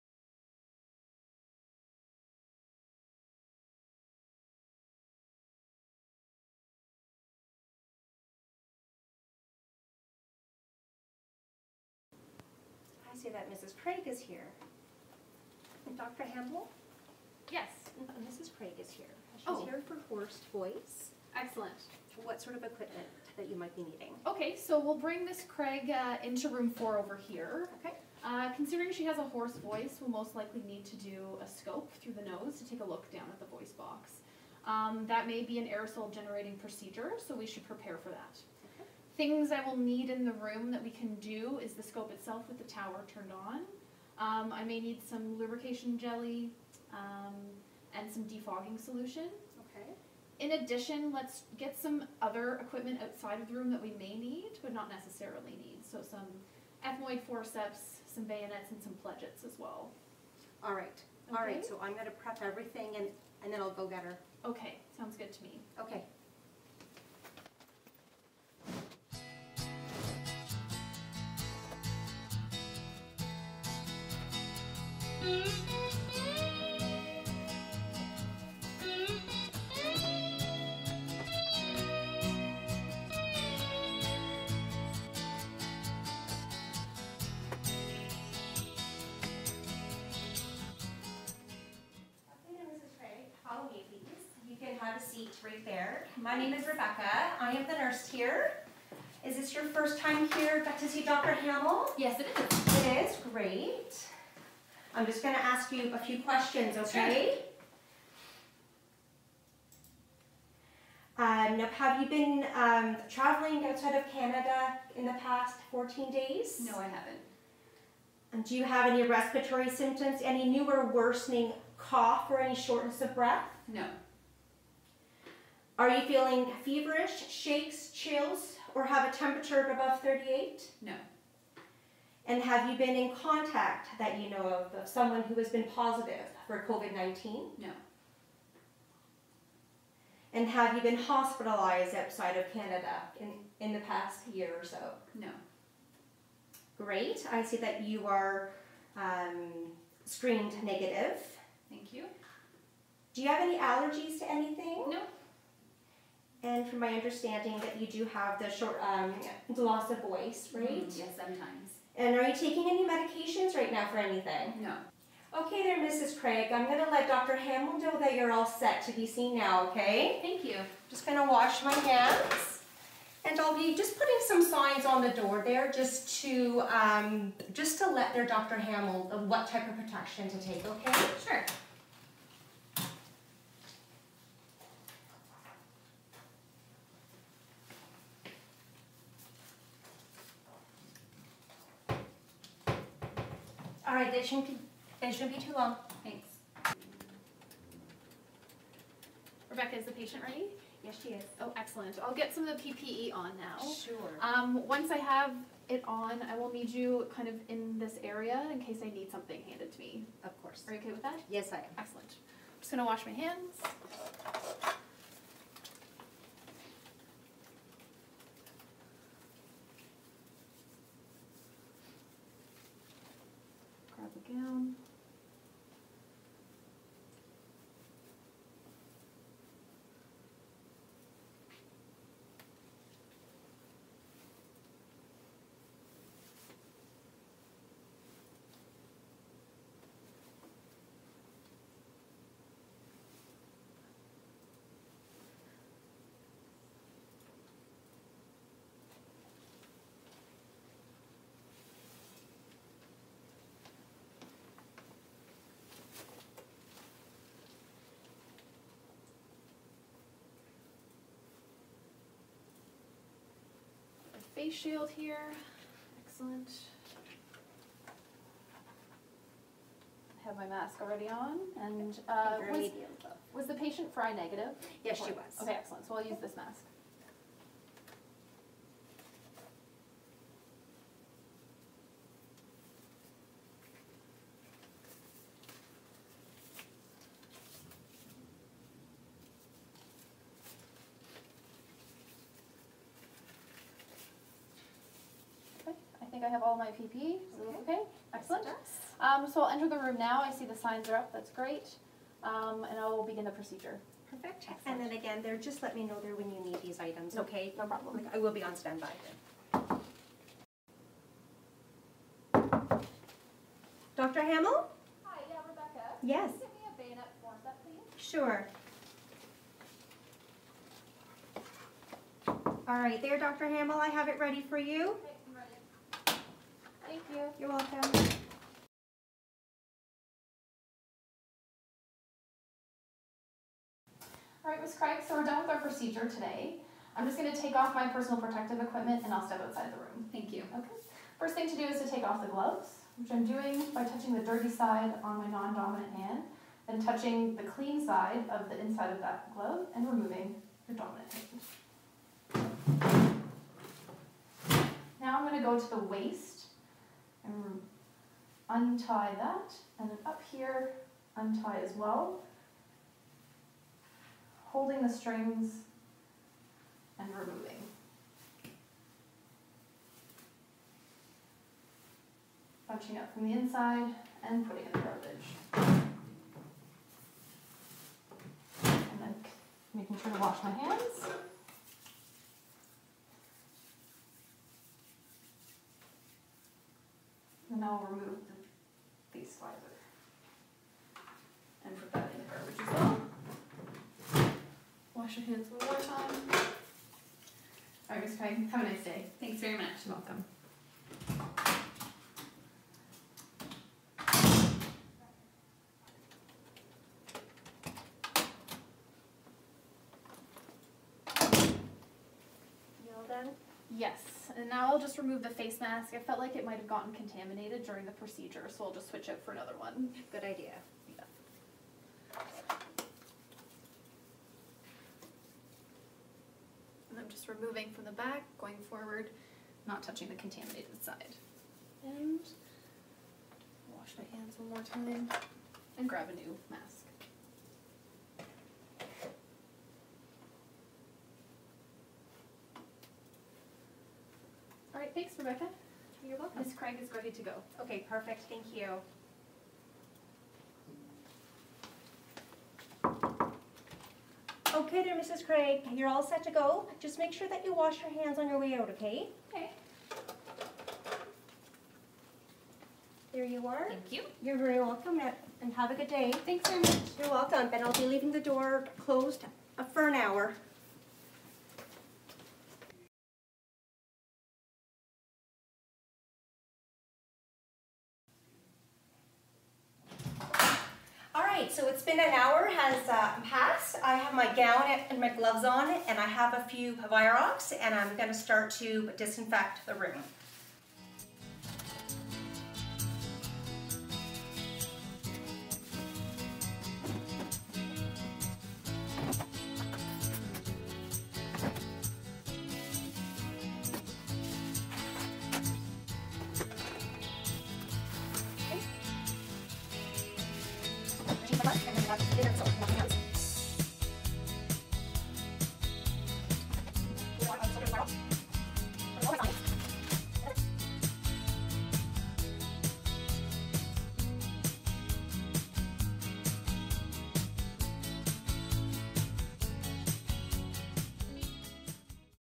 Ambulatory Care ENT during the COVID-19 pandemic
This case was designed during the COVID-19 pandemic in order to improve team preparedness for safely and effectively caring for patients in the out-patient setting.

Learning Objectives:
- Demonstrate aerosol generating procedure workflow in an ENT clinic
- Donning and doffing PPE
- Minimizing people and equipment in room
- Minimizing opening of door during procedure

Authors: Jennifer Dale-Tam, Rebecca Bernal, and Alexandra Craig

Actors:
Dr. Shannan Hamel, MD (as the Otolaryngologist)
Ms. Rebecca Bernal, RPN (as the Nurse)
Ms. Alexandra Craig, RPN  (as the Patient)
Department of Otolaryngology – Head and Neck Surgery, The Ottawa Hospital and the University of Ottawa, The Ottawa Hospital

Director: Jennifer Dale-Tam, Simulation and Nurse Educator, University of Ottawa
Videographer: Kaitlin Endres, Sim Technician, University of Ottawa
Edited by Ms. Kaitlin Endres

With support from:
Dr. Glenn Posner, Medical Director, TOH Simulation Patient Safety Program
Dr. Jerry Maniate, TOH VP Education & Dr. Janet Corral, Associate Dean, Curricular Affairs, University of Arizona

Case:
You are the otolaryngologist in clinic and your next patient is 36-year old Ms. Craig who is here for an assessment of chronic hoarse voice (ongoing for the past 2 months). She will likely require assessment with nasopharyngoscopy (an aerosol generating procedure).

She has no travel history outside of Canada in the past 2 weeks and no hospitalizations outside of Canada in the past year. She has not been in contact with anyone who has tested positive for COVID-19. She denies fever, cough, rhinorrhea, chills or shortness of breath.

 She is otherwise healthy, with no allergies, medical conditions or daily medications. She has a 32 pack-year of smoking (2PPD for the past 16 years).